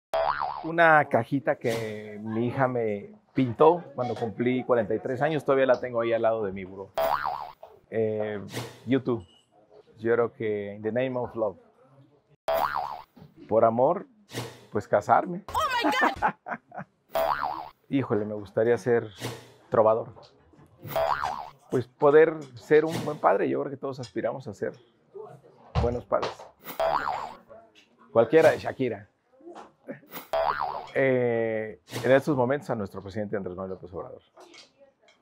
Interrogando a Mario Delgado
Le hicimos preguntas a Mario Delgado presidente nacional de morena para conocerlo un poco más detrás de la política.